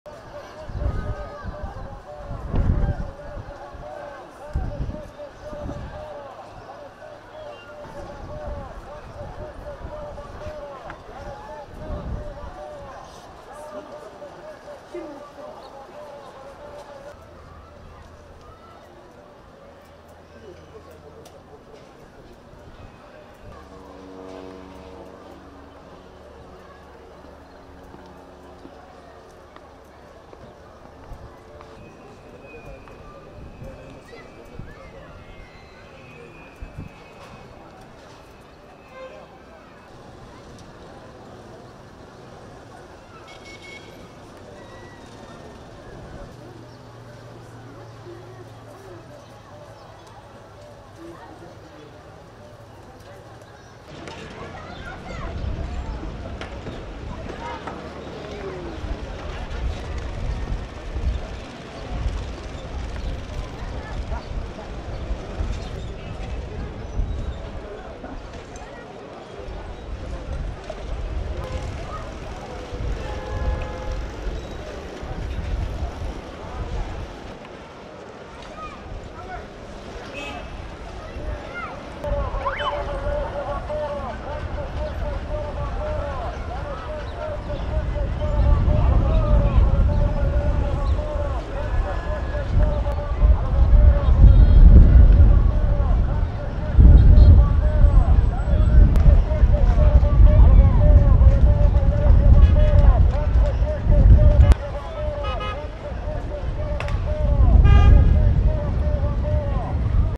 Gaza, l'esercito israeliano bombarda Khan Yunis
Le immagini da Rafah mostrano colonne di fumo che si alzano nel sud della Striscia